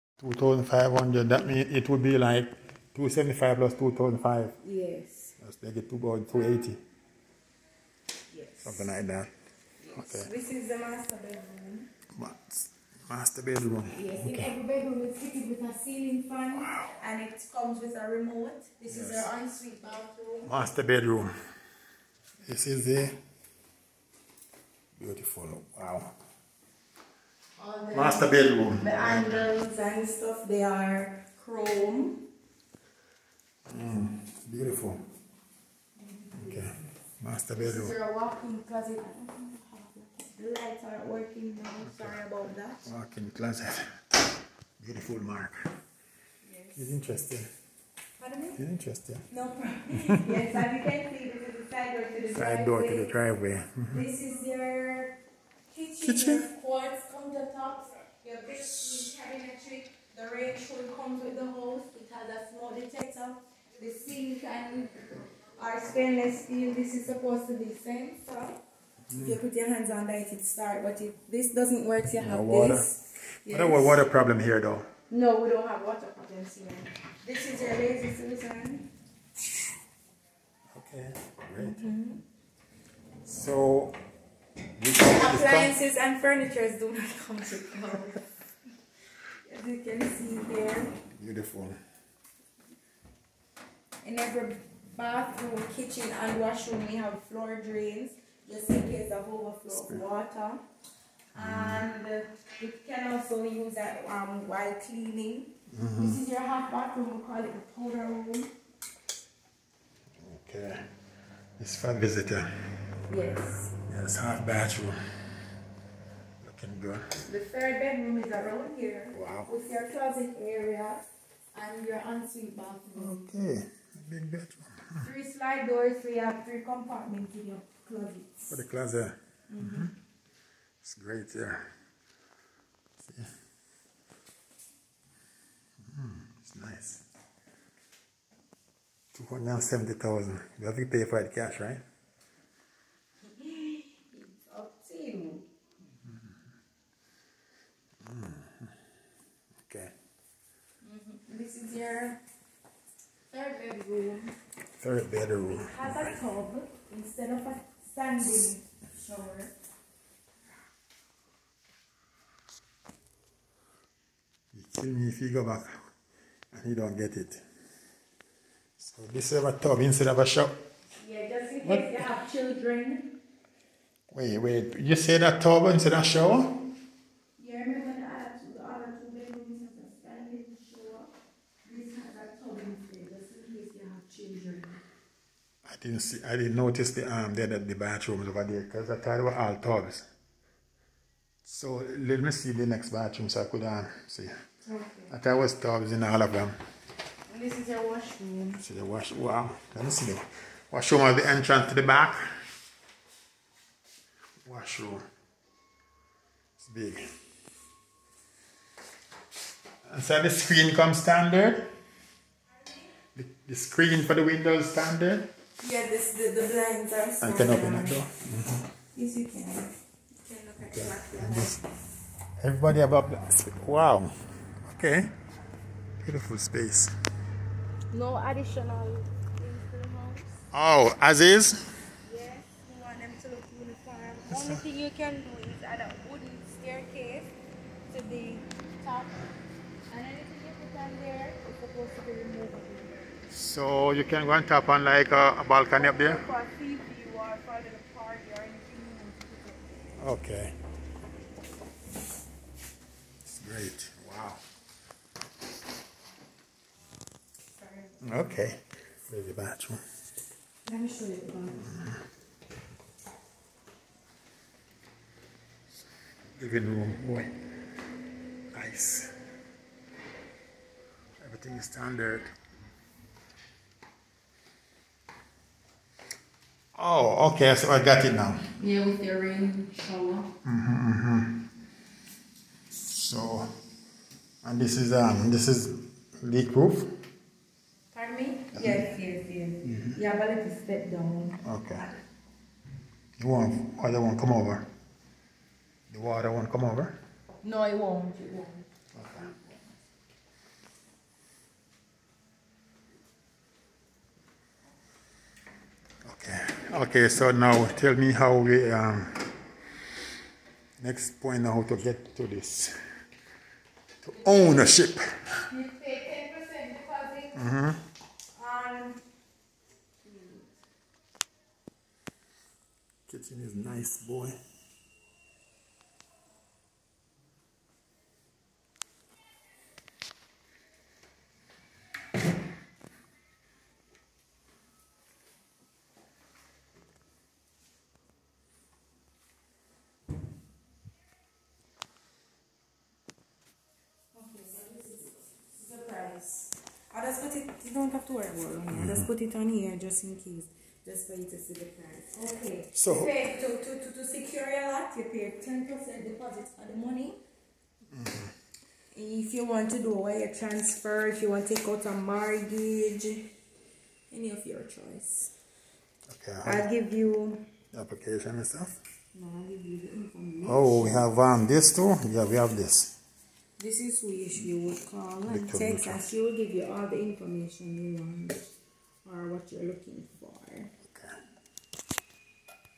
Pyramid Pointe, Ocho Rios, Jamaica🇯🇲
Taking a tour through the 3 bedroom US$270,000 completed unit.